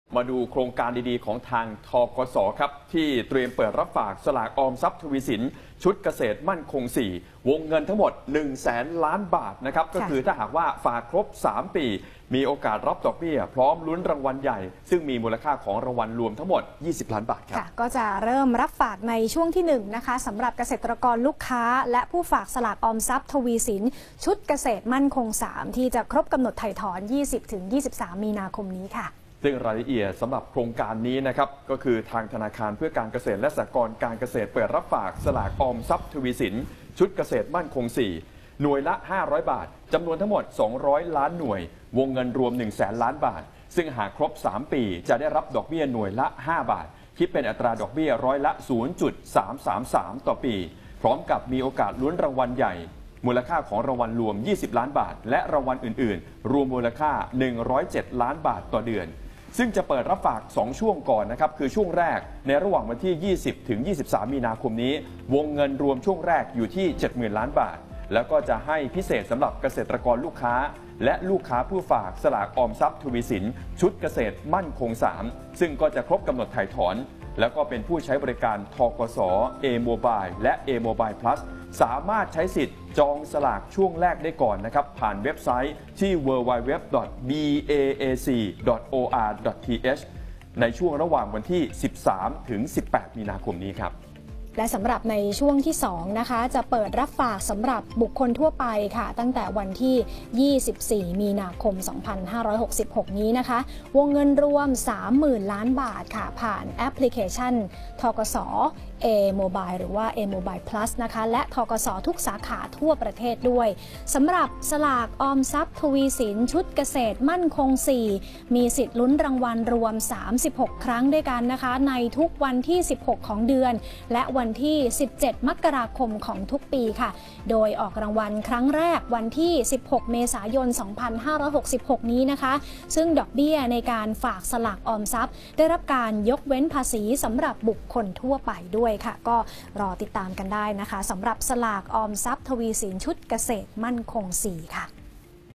ธ.ก.ส. เปิดรับฝาก “สลากออมทรัพย์ทวีสิน ชุดเกษตรมั่นคง 4” ลุ้นรางวัลใหญ่ 20 ล้านบาท
ธ.ก.ส. เปิดรับฝาก “สลากออมทรัพย์ทวีสิน ชุดเกษตรมั่นคง 4” วงเงิน 100,000 ล้านบาท ฝากครบ 3 ปี รับดอกเบี้ย พร้อมลุ้นรางวัลใหญ่ 20 ล้านบาท เริ่มรับฝากช่วงที่ 1 สำหรับเกษตรกรลูกค้า และผู้ฝากสลากออมทรัพย์ทวีสิน ชุดเกษตรมั่นคง 3 ที่ครบกำหนดไถ่ถอน 20-23 มีนาคมนี้